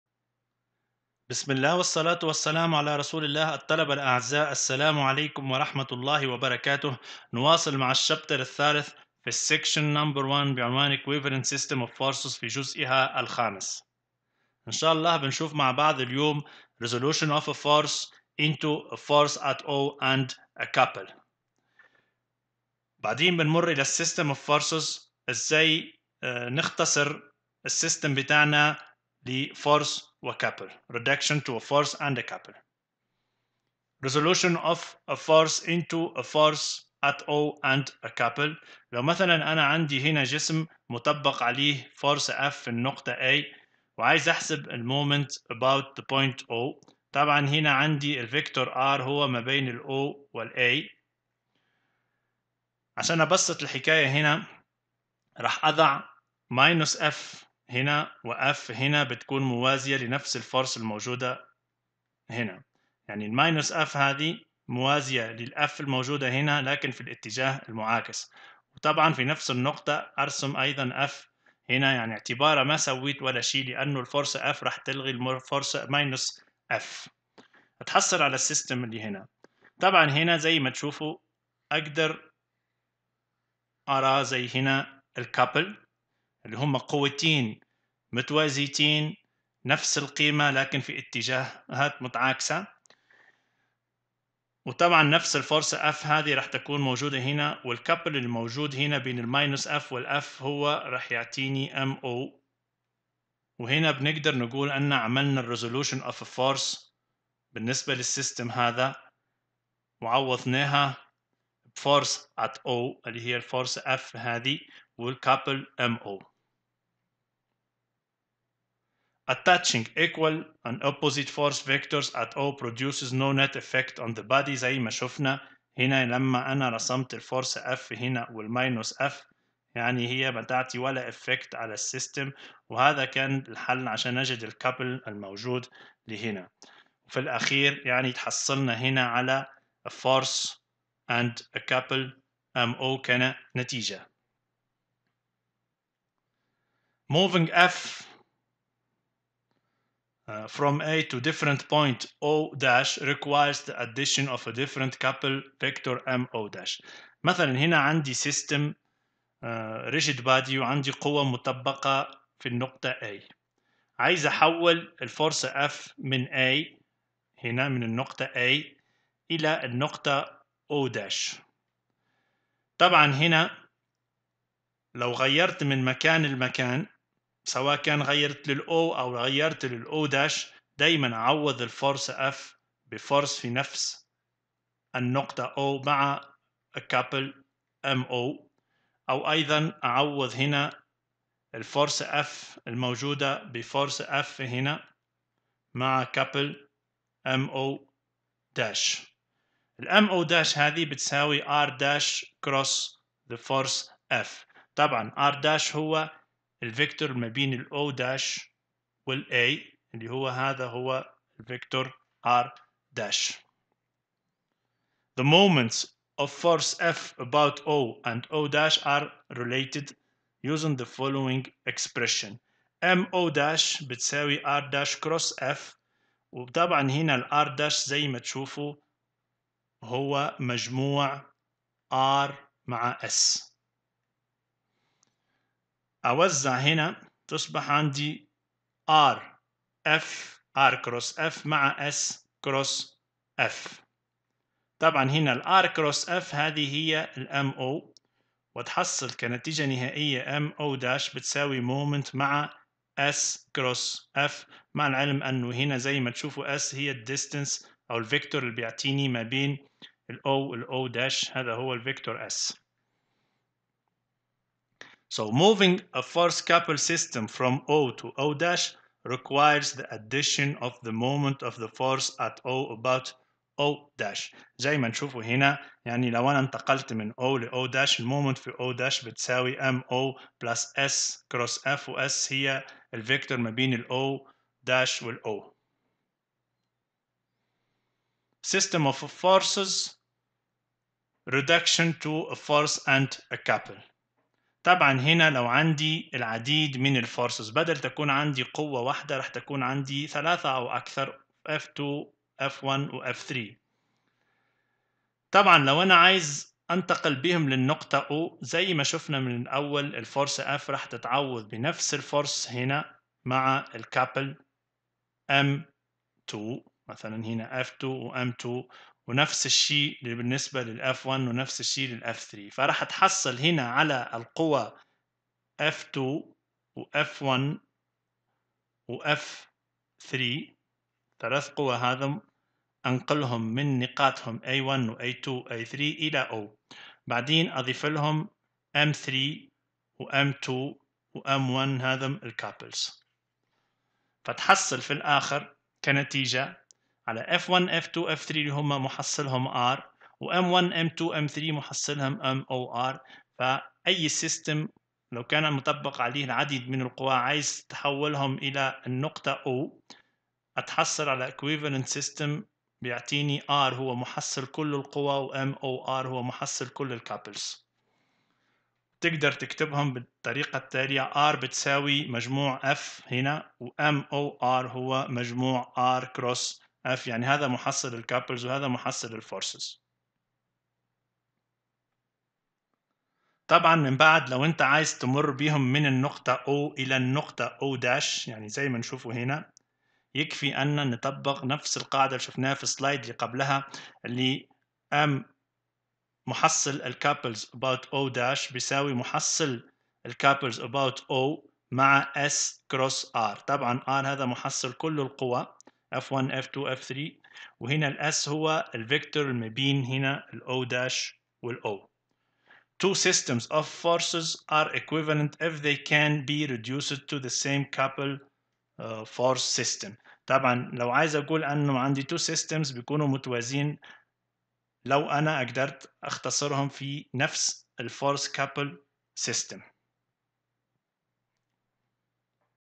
STATICS | 3-9 | Equivalent System of Forces
الوحدة 3 : Statics of Rigid bodies
الدرس 9 : Equivalent System of Forces
• Resolution of a Force Into a Force at O and a Couple
• System of Forces:  Reduction to a Force and a Couple